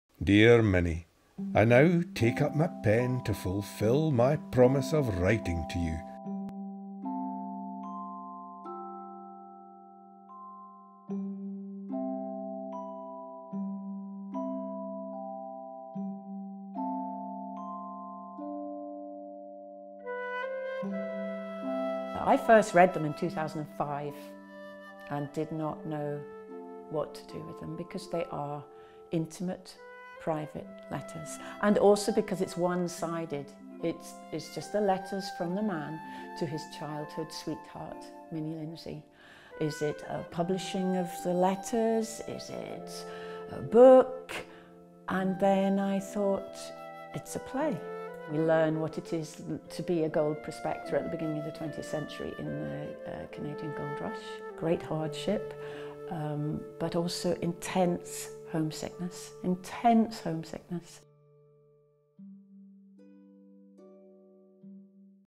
Love letters from gold hunter found in sewing box after 100 years
Love letters hidden in a museum for more than a century have inspired a new play.

Miss Lindsay’s Secret was written after the notes were discovered inside a sewing box at Glen Esk Museum in Angus.

Alexander Middleton left the area in 1902 to seek his fortune in the Canadian Klondike goldrush, leaving behind his childhood sweetheart, seamstress Minnie Lindsay.

His correspondence to her was uncovered more than 100 years later by actor and playwright Maria MacDonell.

“A first I didn’t know what to do with them because they are intimate, private letters, and also because they are one-sided,” she said.

“I thought ‘is it a talk, is it a book?’ then decided it should be a play.”

Set in the museum, the story is told through the eyes of the museum curator, who will be played by Maria at the Edinburgh Fringe in August and Aberdeen’s Lemon Tree in November.